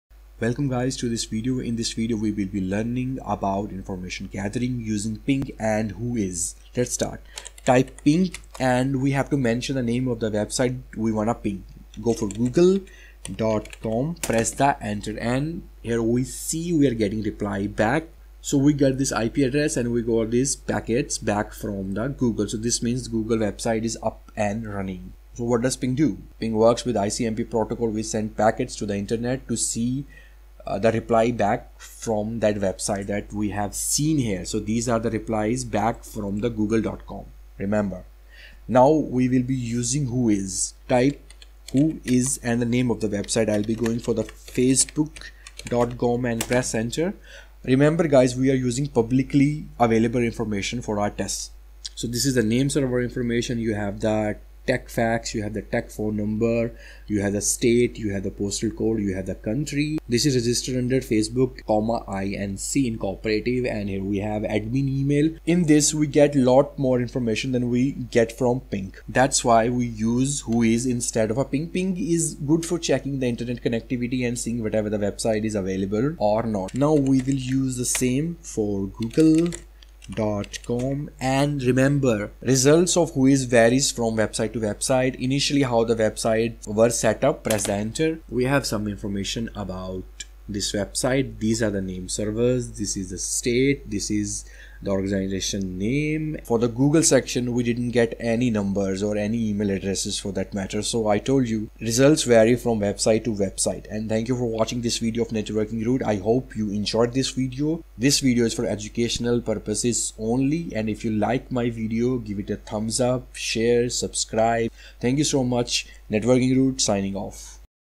Information Gathering using Whois and ping in Kali Linux. Hands on practical lab lecture 5.
in this video you will learn how to collect information on your target using whois and ping command. these came preinstall in kali linux. its an example of active data collection. this video is for educational purposes only.